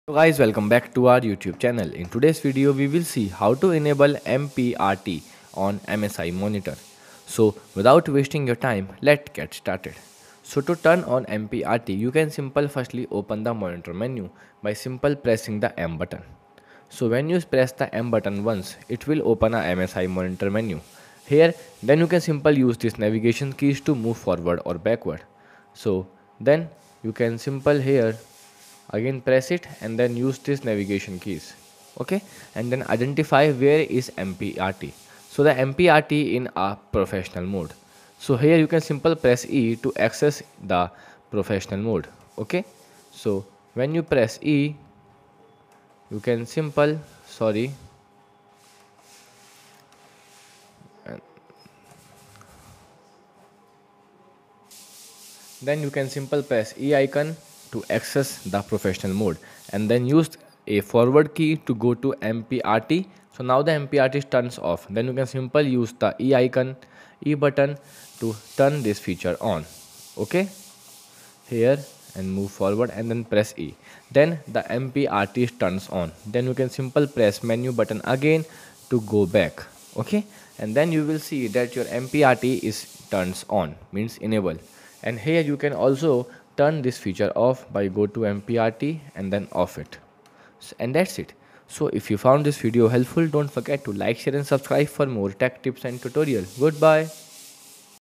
How To Enable MPRT On MSI Monitor
In this informative video, we will guide you through the process of enabling the MPRT (Motion Picture Response Time) feature on your MSI monitor. MPRT is essential for reducing motion blur and enhancing your gaming experience.